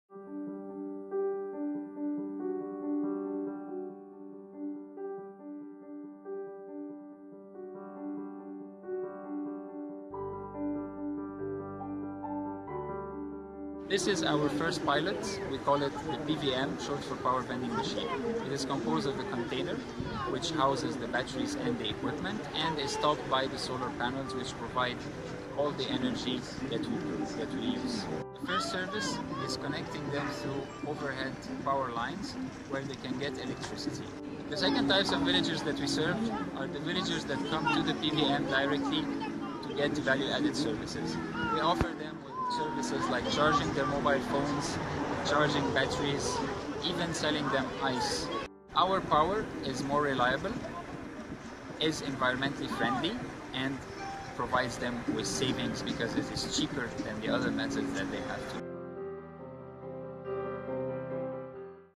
Solera PVM introduction
Solera has implemented its Power Vending Machine (PVM) pilot project in Luangwa Bridge Market, Zambia (Dec, 2018). This is a short introduction describing this innovative rural electrification solution.